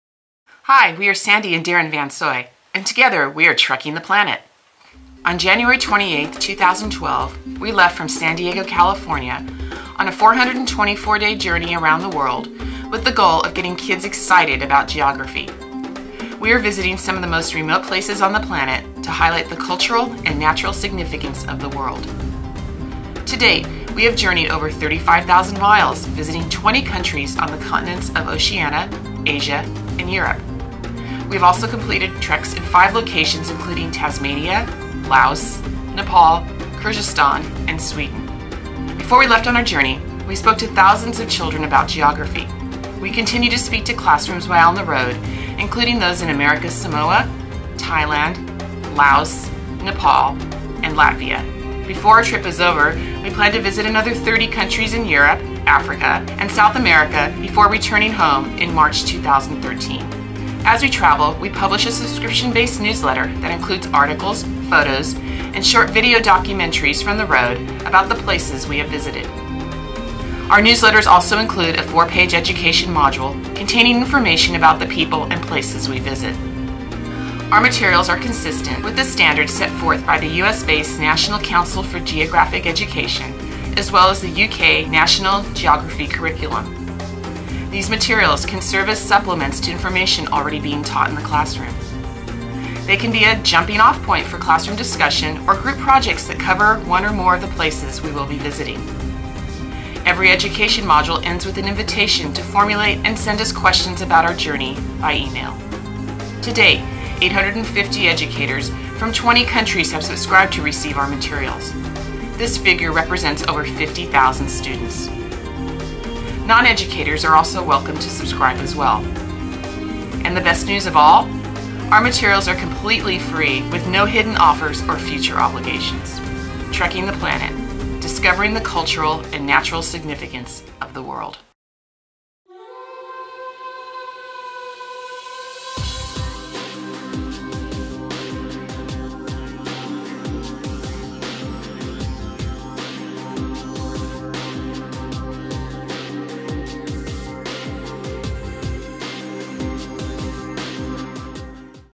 Trekking the Planet: Two-Minute Overview (Update)
Our two-minute overview video has been updated!  In this video you will learn about our journey and see some of our pictures and video from our school visits and treks. During our expedition, we are traveling to 50 countries on six continents over 424 days or 14 months. We are taking this trip in the hope of getting kids excited about the subject of geography.  Each week we are making geography accessible to them by publishing educational materials from the road.  To date, we have received 850 subscriptions from educators located in 20 countries, representing 50,000 students. We also have a "Friends" subscriber base (representing our friends and family plus others who just want to come along for the ride).

During our trip, we are taking a series of multiple day hikes or treks to some of the most remote and unspoiled places on the planet.  Each location was selected because of its natural or cultural significance.  We have already completed five treks, hiking over 200 miles.  When we can, we focus on sustainability practices, celebrating the successes that we find as we travel.   We are also staying "close to the ground" meaning that, instead of flying, we try to use local public transportation whenever we can, whether it is a bus, a train, a ferry or a ship.  To date, we have covered over 36,000 miles, with more than 80% of that on land or sea.